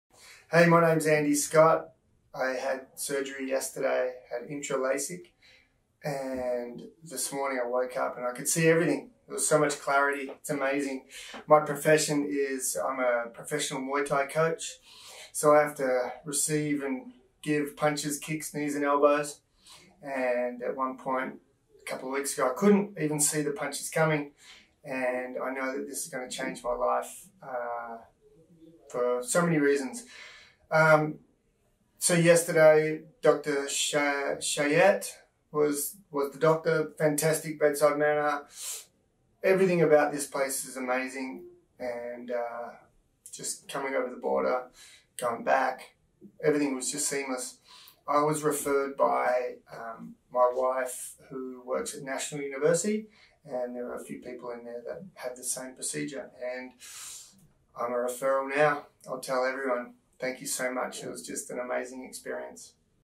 IntraLASIK Surgery Testimonial - Andrew Scott - CODET Vision Institute
CODET Vision Institute

“I´m a professional Muay Thai Coach and a couple of weeks ago I couldn't even see the punches coming”

Andrew Scott, Muay Thai Coach – Cardiff, CA
IntraLASIK Surgery with Dr. Arturo Chayet.

Avenida Padre Kino # 10159 Zona Rio Tijuana B.C. C.P. 22010